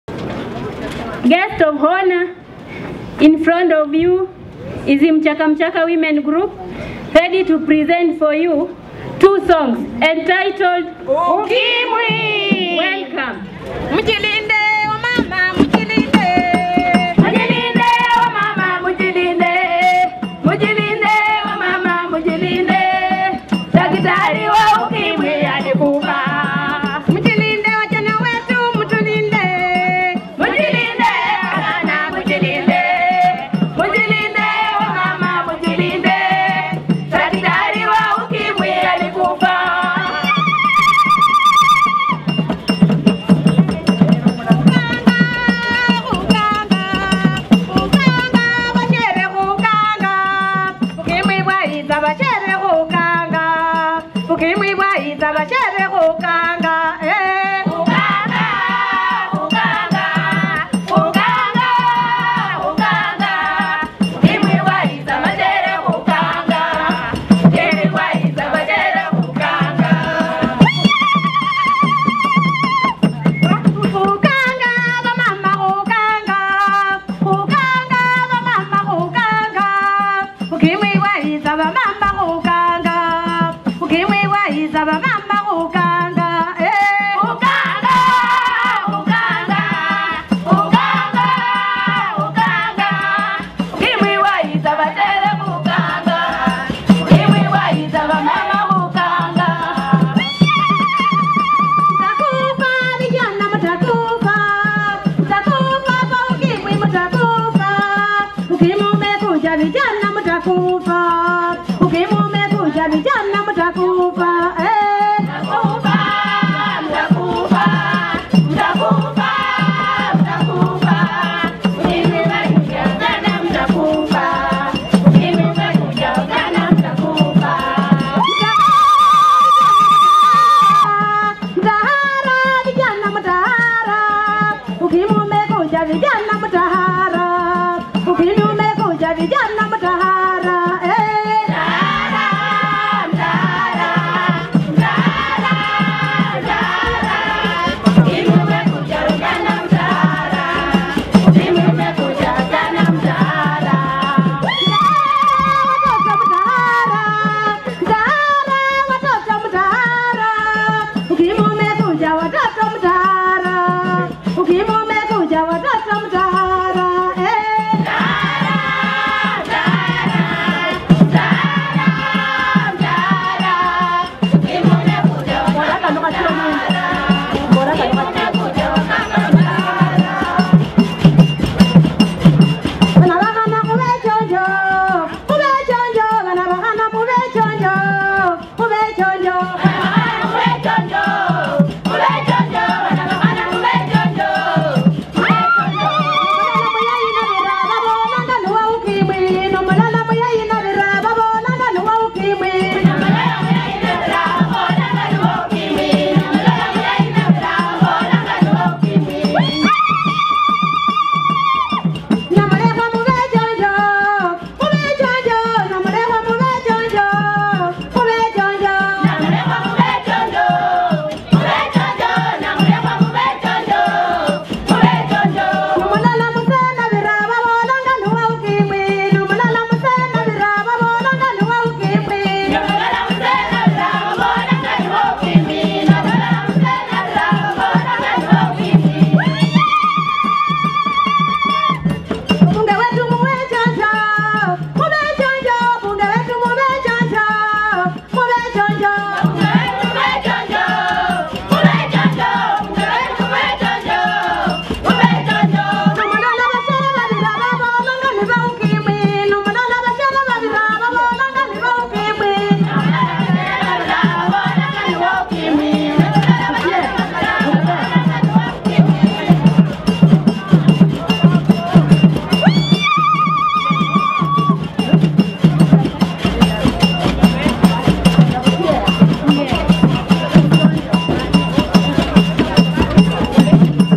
Women's Group Sings 2 Song in Kenya
Filmed in Kakamega, Kenya.